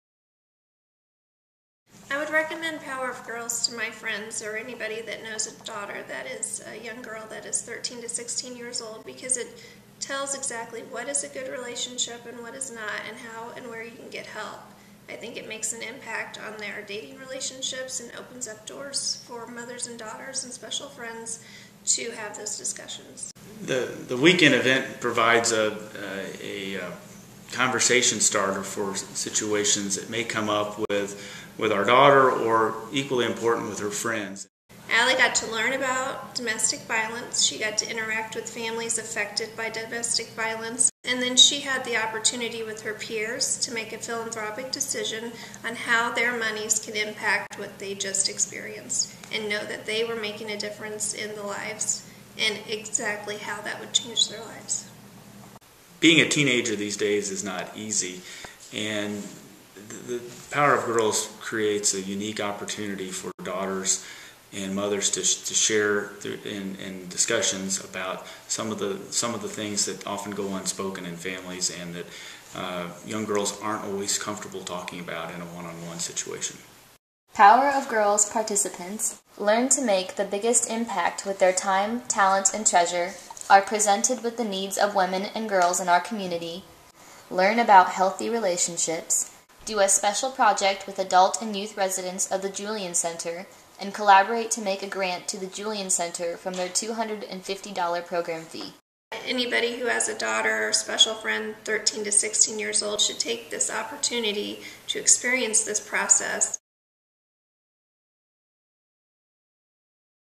Power of Girls 2011
Register now for Power of Girls 2011, a day-long healthy relationships program for girls age 13-16 and their mother or a special woman in their life.